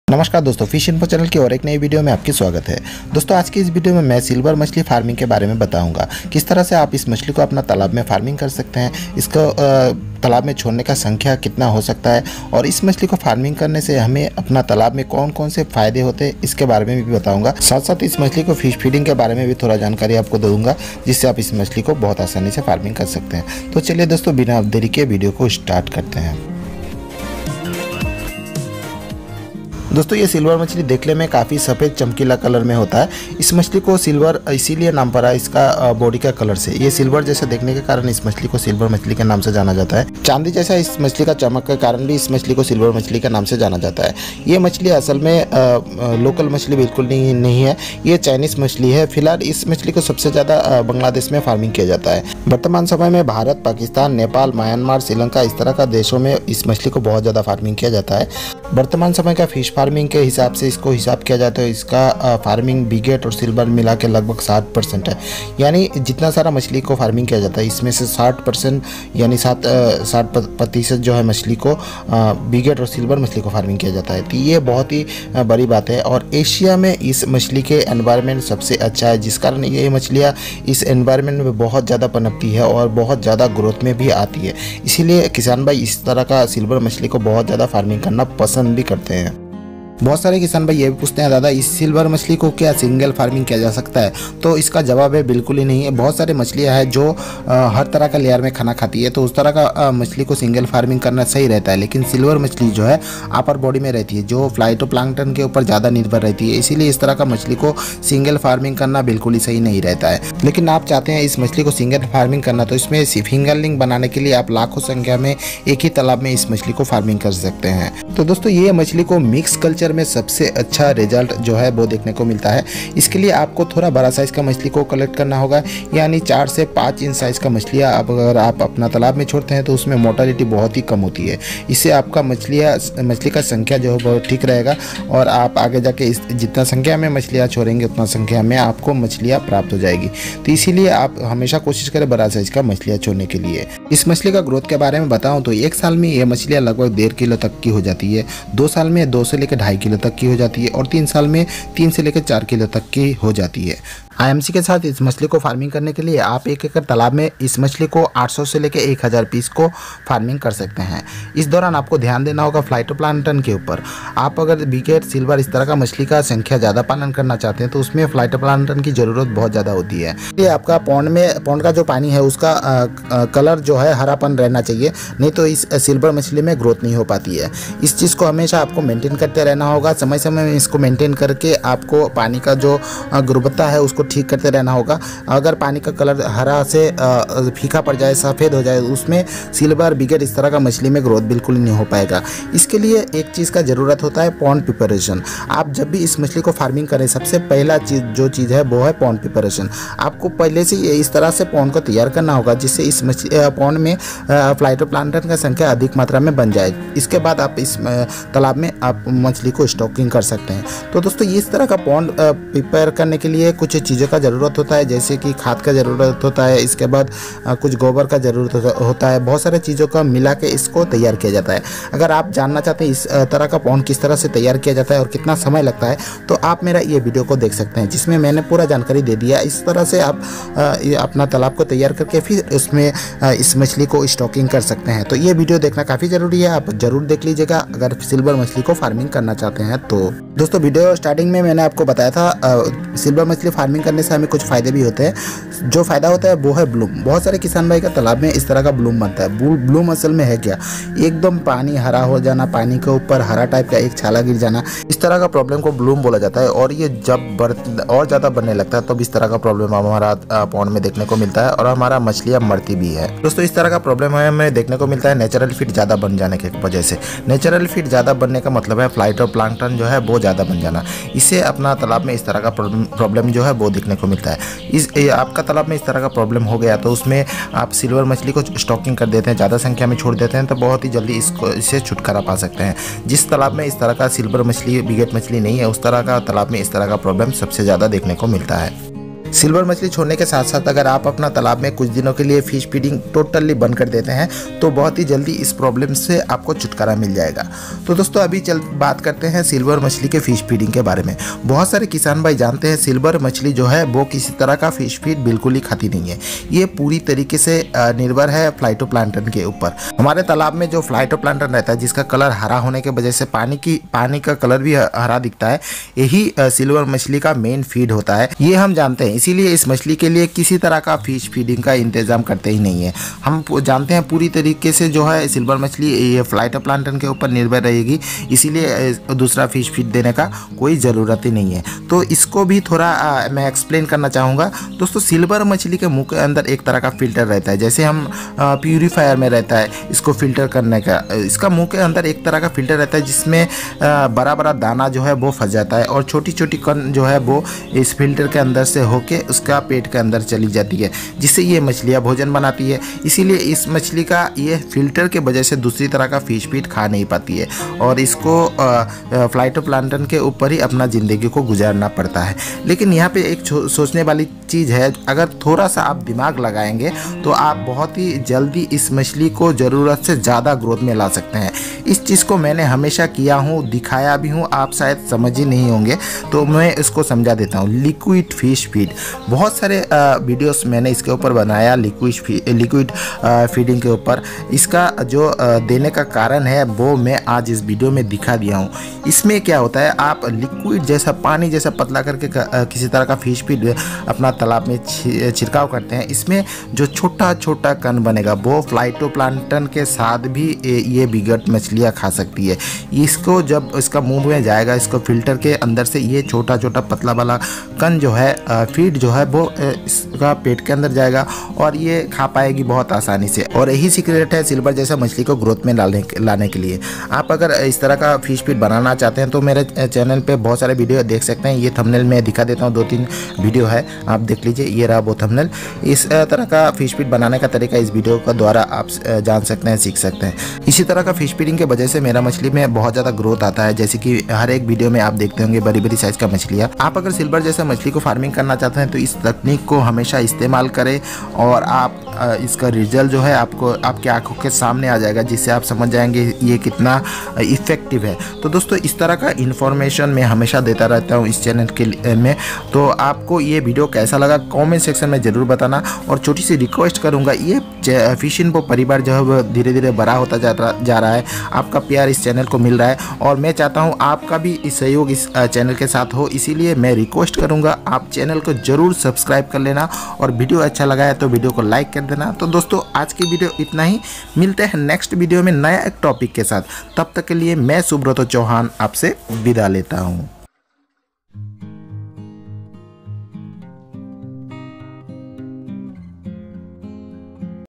best silver carp fish farming method | big head fish farming | how to growth my fish
video topic:- silver carp fish farming in india,silver carp fish mixed farming,phytoplankton eating fish,silver carp fish feeding

Silver carp fish is required for mixed farming in 1 acre pond- 800/1,000 pieces

Our project Name:-
M/s Ganga Matshya hatchery
address:-Purbo Collegepara, (Jalkhana More)Raiganj, uttar Dinajpur